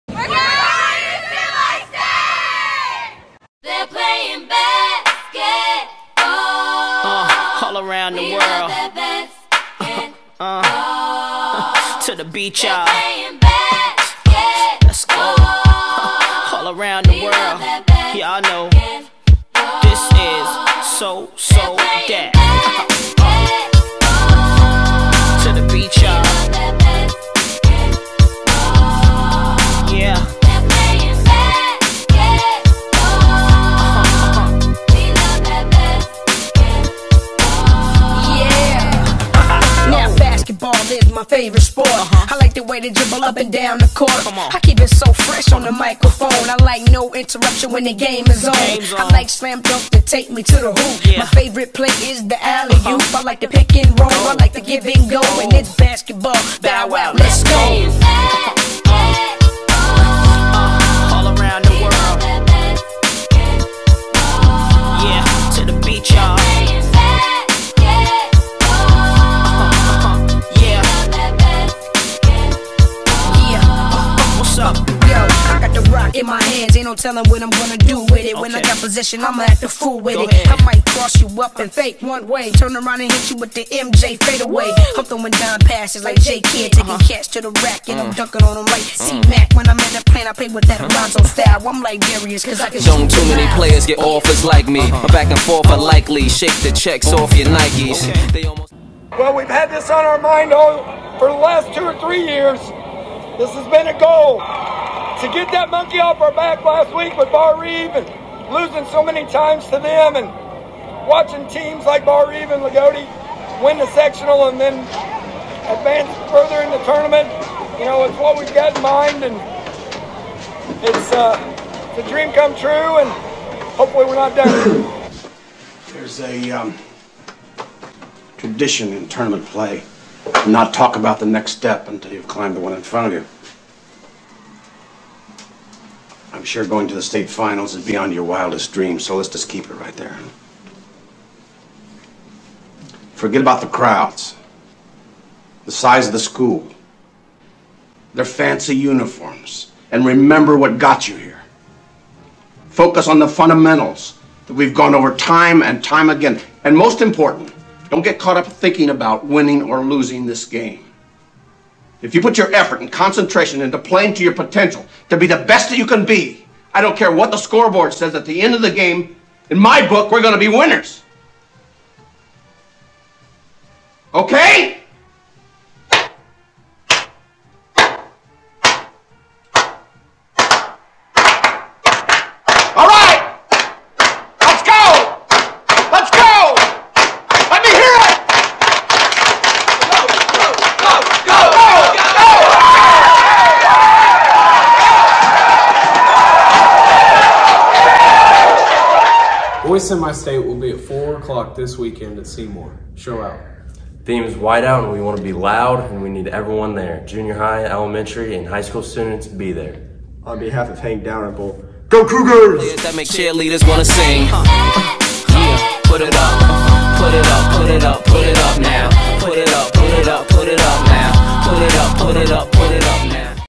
2022 North Daviess Semi-State Promo
Cougar News Network and the North Daviess High School Yearbook created this promo using video footage from John Burkhart, The IHSAA Chamions Network, and WTHI, also using photos from S. Willis Photography. Good Luck to the Cougars in Semi-State!